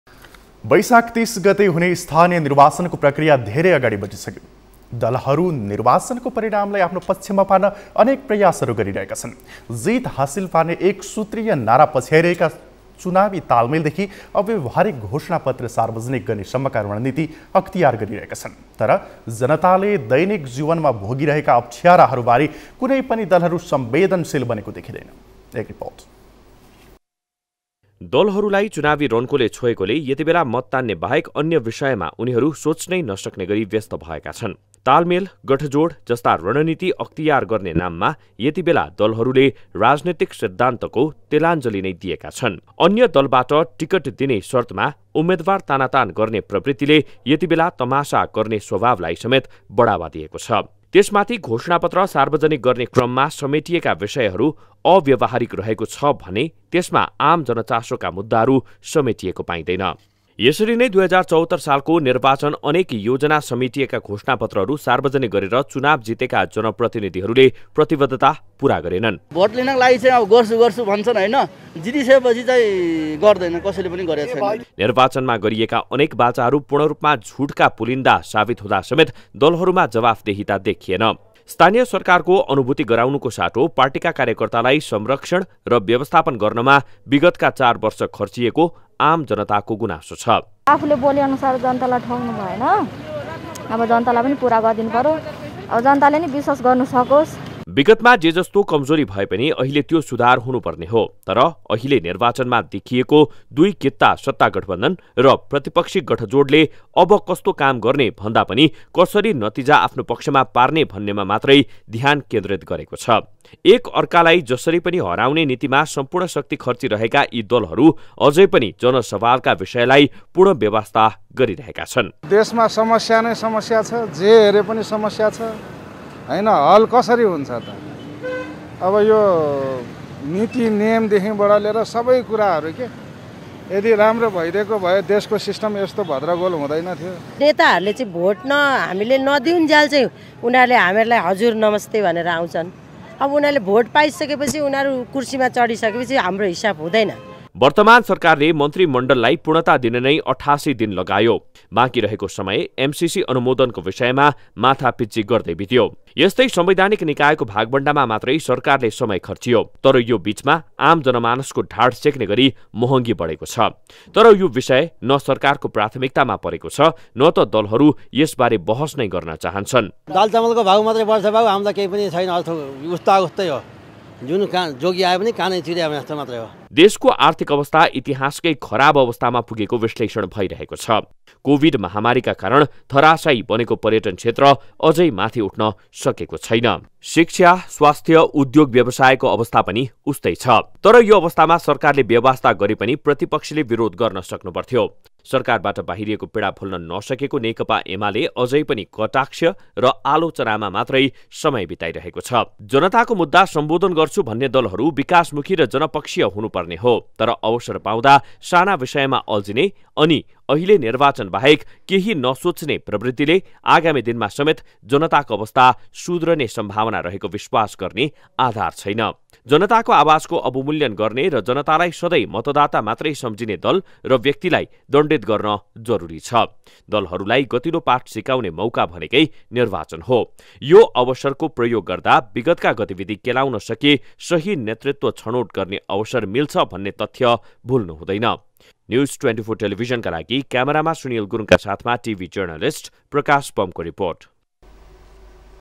निराश मतादाताको स्वर– ‘जनताको सवालमा ध्यान कहिले दिने ?’ - NEWS24 TV
बैशाख ३० गते हुने स्थानीय निर्वाचनको प्रक्रिया धेरै अगाडि बढिसकेको छ । दलहरु निर्वाचनको परिणामलाई आफ्नो पक्षमा पार्न अनेक प्रयास गरिरहेका छन् । जित हासिल पार्ने एकसूत्रीय नारा पछ्याइरहेका चुनावी तालमेलदेखि अव्यवहाकरिक घोषणा पत्र सार्वजनिक गर्नेसम्मका रणनिति अख्तियार गरिरहेका छन् । तर जनताले दैनिक जीवनमा भोगिरहेका अप्ठ्याराबारे कुनैपनि दलहरु संबेदशिल बनेको देखिदैन ।